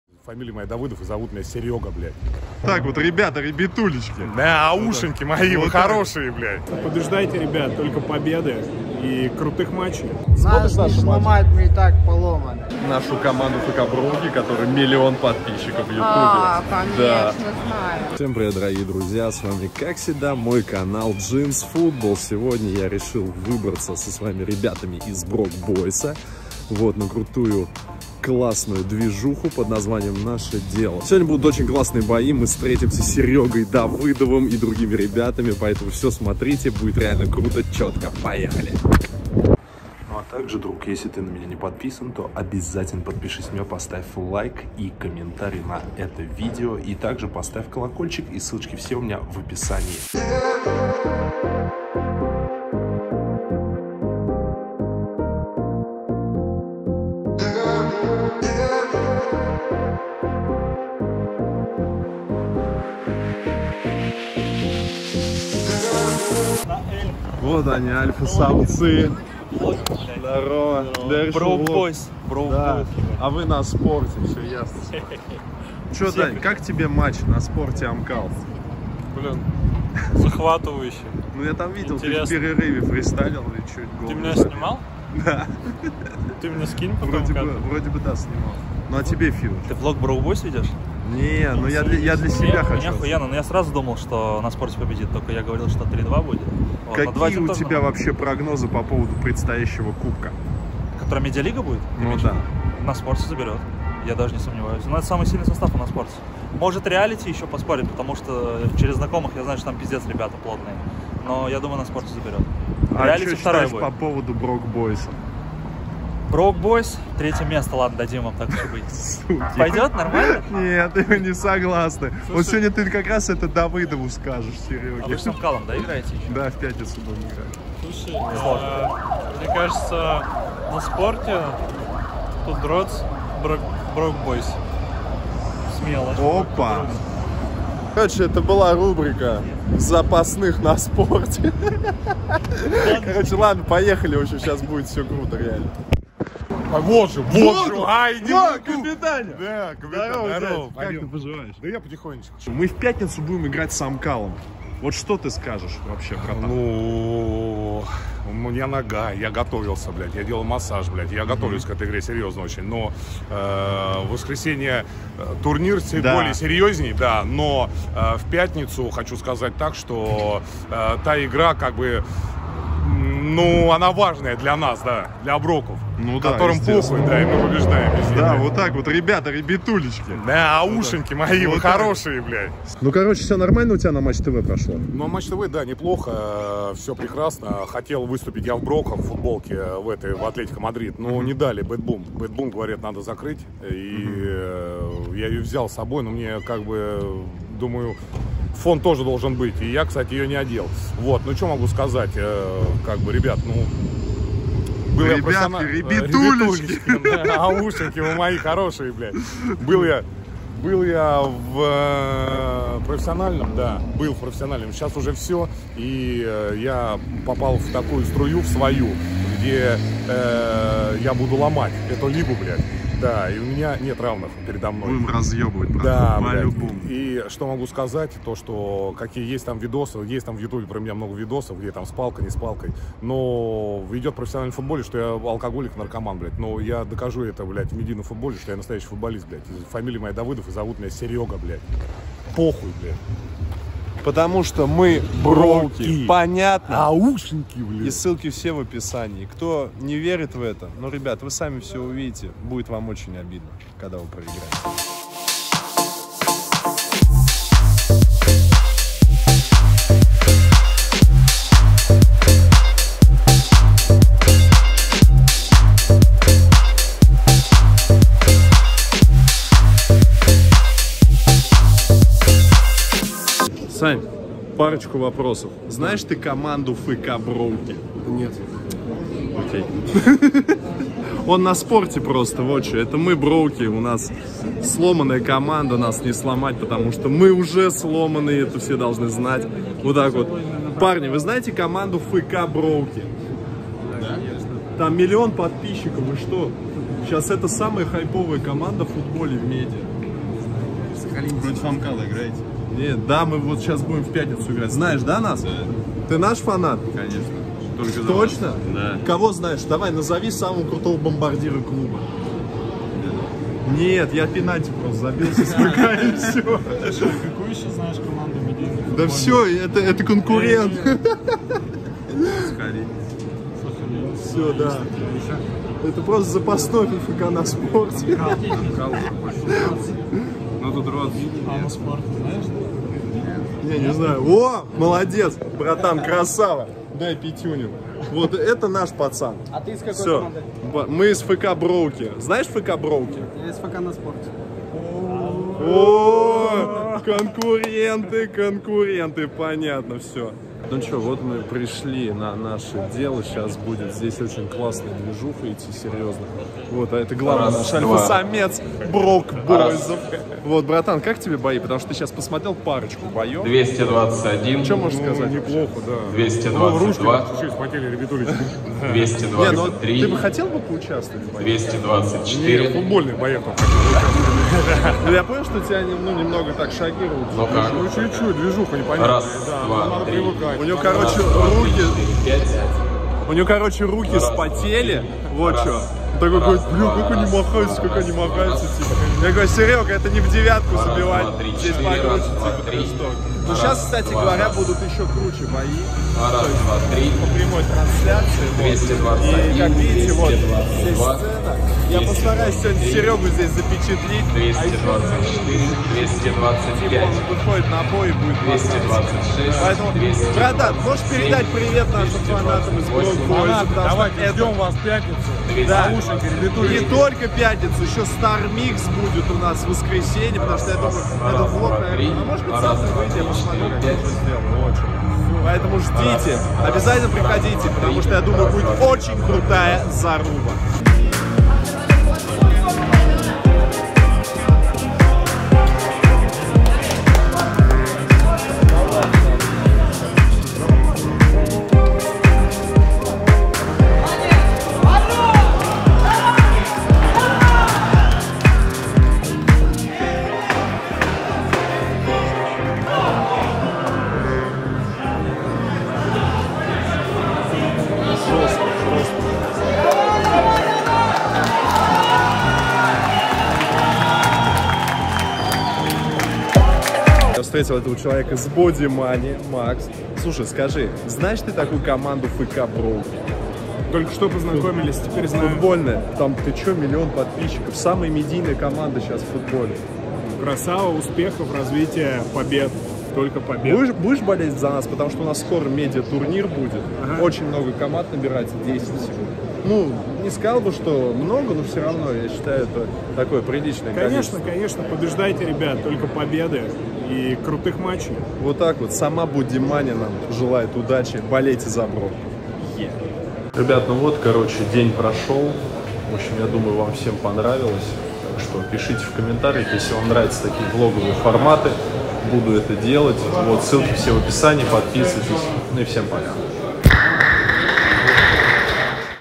ДЕНЬ с СЕРГЕЕМ ДАВЫДОВЫМ из ФК БРОУКИ
Всем привет мои дорогие друзья!
С вами Артурка, и  вернулся с крутыми новостями. В этом выпуске вы увидите легенду фк Броуков Сергея Давыдова. Мы решили пойти с ним на бои Наше Дело , заодно и пообщаться в жизни о спорте и броуках.